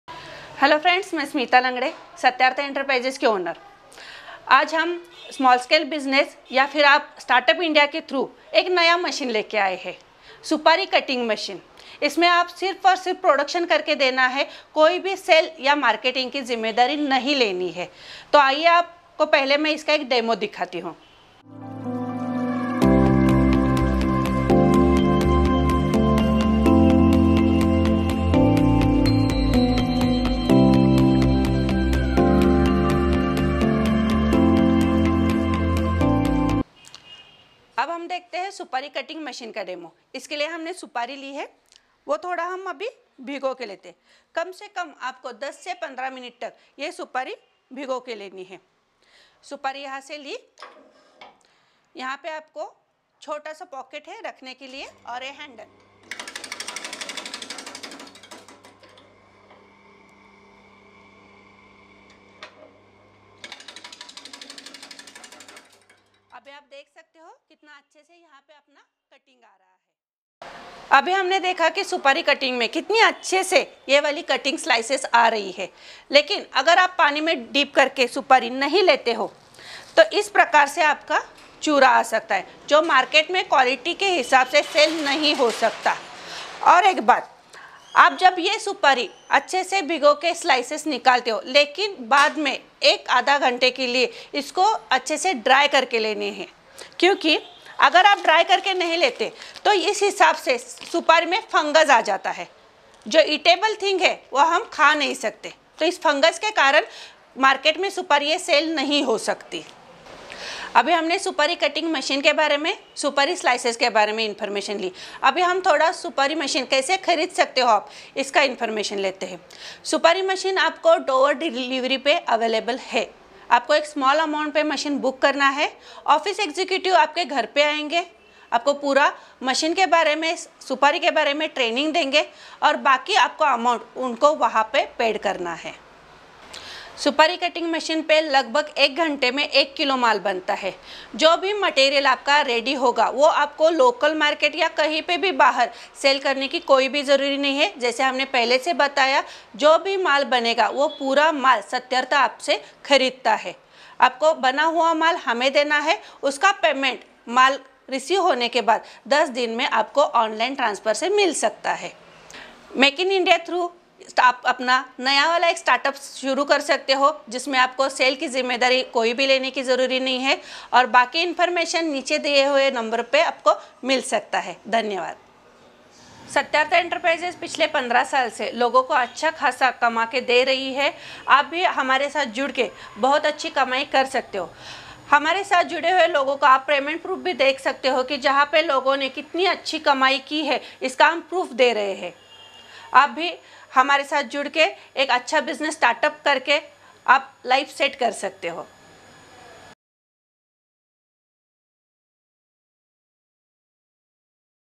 खुद का Business start करे Supari Cutting Machine (Advanced +Fast) के साथ | Start your Home Business
"Are you looking for a profitable business opportunity? Introducing our high-quality Supari (Betel Nut) Cutting Machine, perfect for individuals who want to start their own venture from the comfort of their homes.

Ideal for home-based entrepreneurs, this high-quality machine ensures precision and efficiency. Be your own boss and start your venture hassle-free from home. Seize the chance to turn your dreams into reality today!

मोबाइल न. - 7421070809

Benefits of the Supari Cutting Machine(Betel Nut):

✅ Individual Production Machine
✅ Job Work Guarantee to All Operators
✅ Home Base & Easy to Operate
✅ No High-level Skill Required
✅ Stamp Paper Agreement
✅ No Required Marketing Knowledge
✅ Buy Back Agreement Available

What is the Buy Back facility?
We offer a unique buy back facility where we purchase the finished products from you, taking care of the marketing aspect